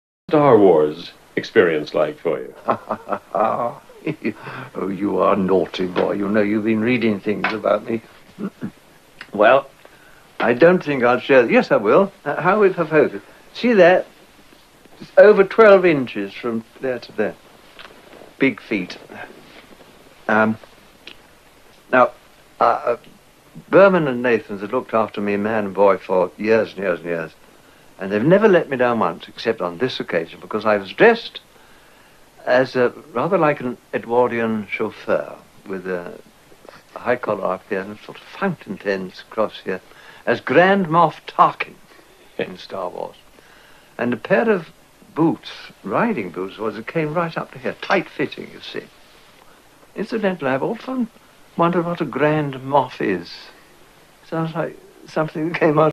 Rare interview - Peter Cushing about Star wars part. 1 (1977)
A rare star wars interview by peter cushing. A great actor tells an anecdote